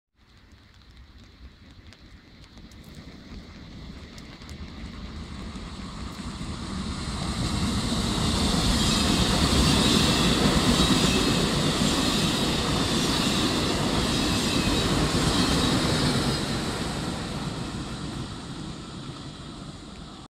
E464 079 Trenord+6 2 piani Trenord e XMPR - Monza Sobborghi - 01/05/2017
R 10787 Milano Porta Garibaldi Bergamo con 6 carrozze a 2 piani (pilota XMPR, le altre Trenord) spinte dalla E464 079 Trenord